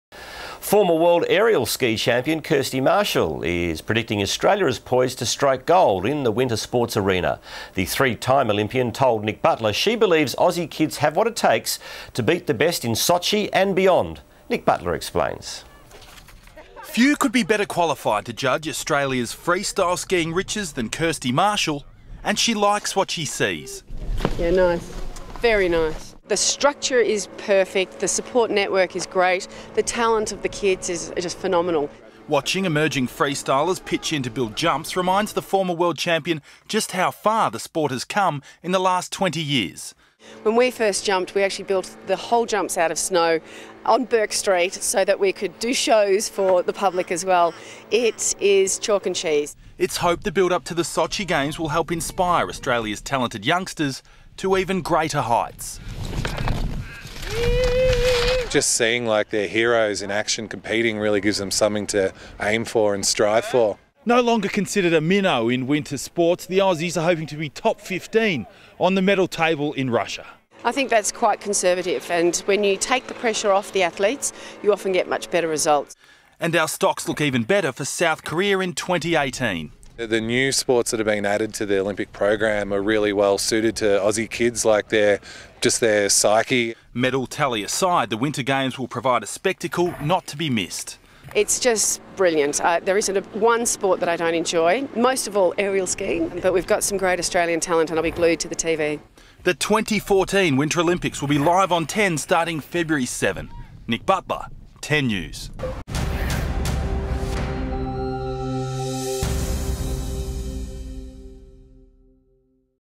Winter winners
Former world aerial ski champion Kirstie Marshall is predicting Australia is poised to strike gold at Sochi during the 2014 Winter Olympics.